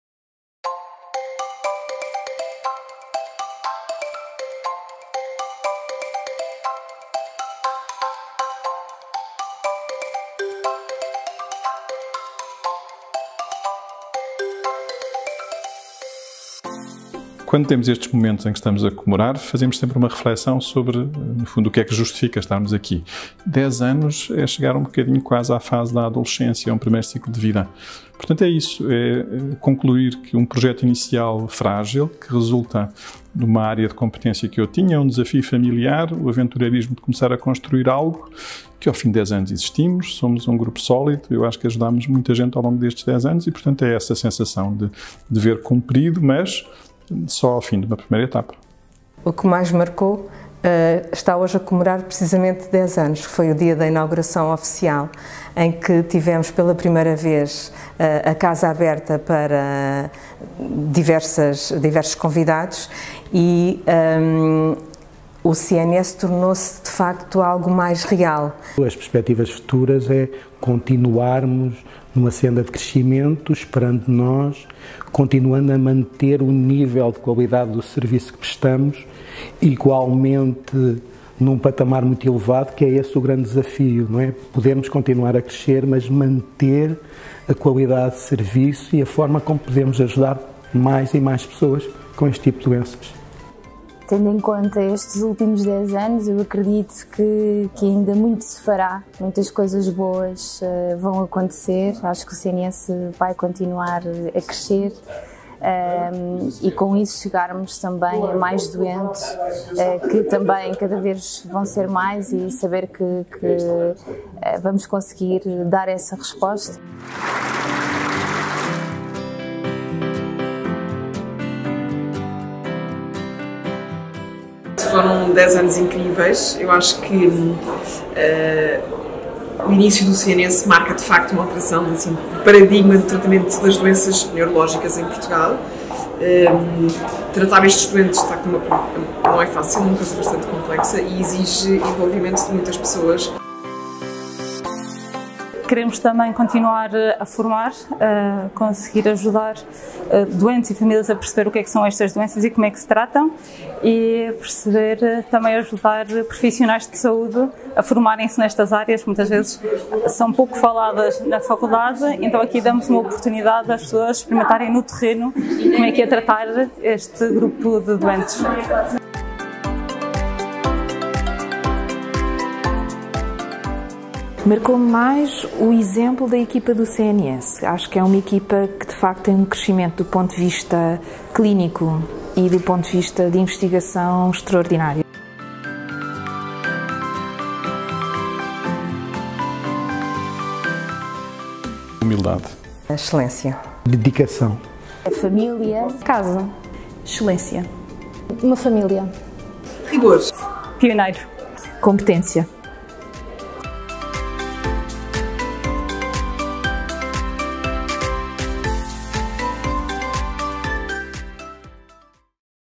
10.º aniversário CNS
Decorreu no dia 27 de janeiro, o evento de comemoração do 10.º aniversário do CNS – Campus Neurológico.

O evento contou com duas conferências que abordaram os temas “O que aprendemos nestes 10 anos?” e “How We Should Treat Neurological Disorders in the 21st Century”, tendo como palestrantes o Prof. Doutor Joaquim Ferreira, Diretor Clínico do CNS – Campus Neurológico, e o Prof. Doutor John Krakauer, professor e investigador no Johns Hopkins School of Medicine, nos Estados Unidos.

Agradecemos a presença de todos!